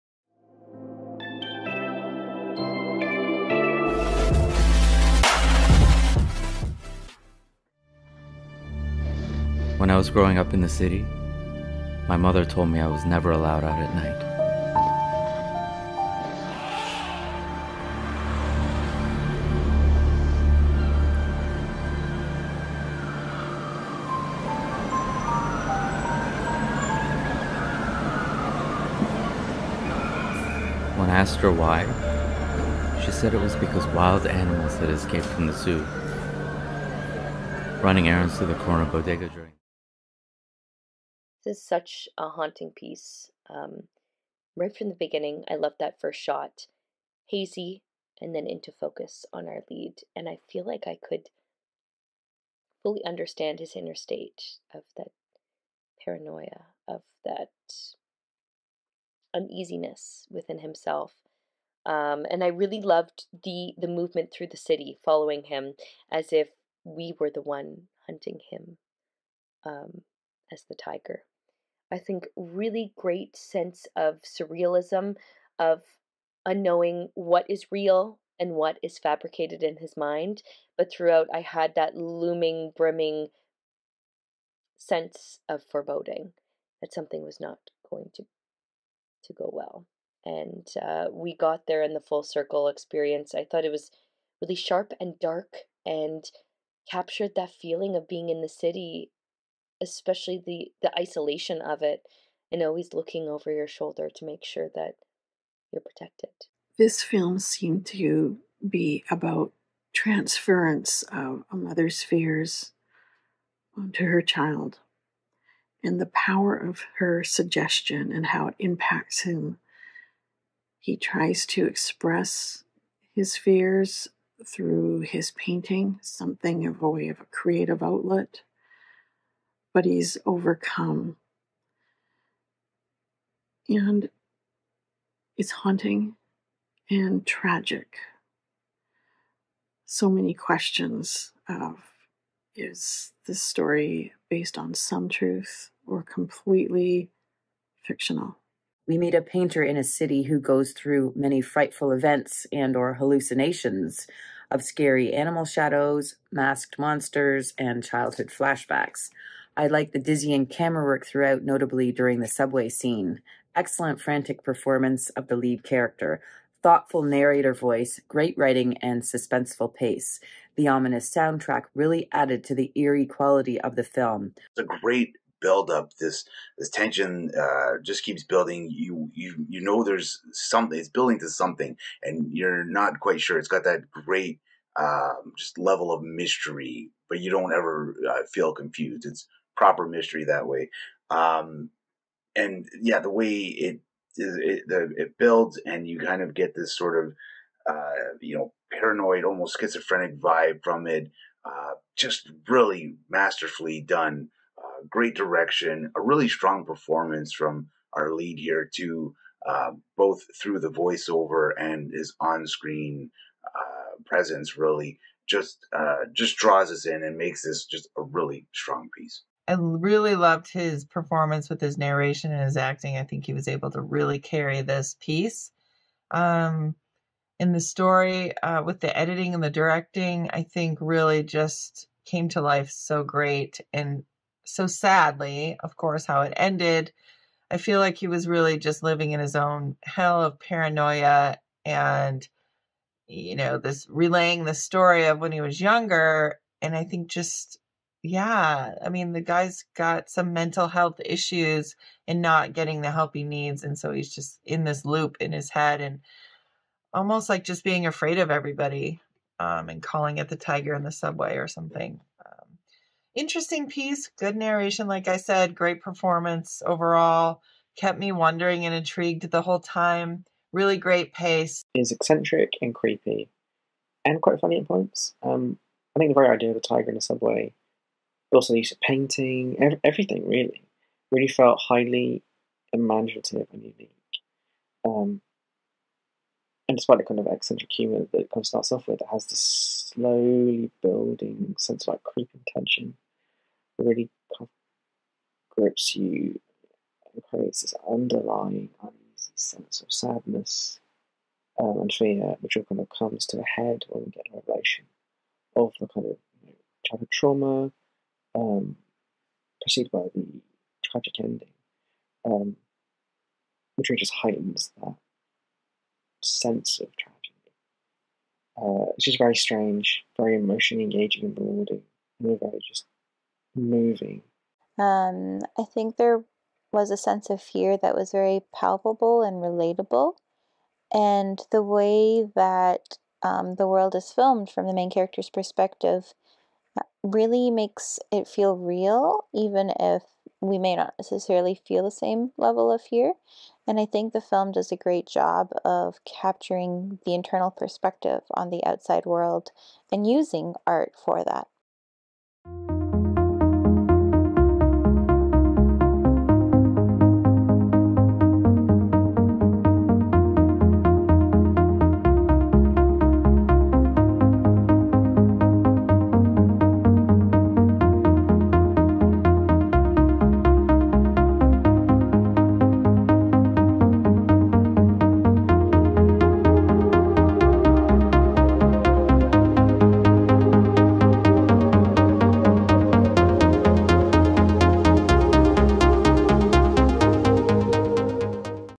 A TIGER IN THE SUBWAY short film, audience feedback March 2023 WILDsound Festival
A TIGER IN THE SUBWAY, 9min,. USA, Thriller/Drama
Directed by James Abrams
An agoraphobic painter reflects on an old white lie his mother told him as a child.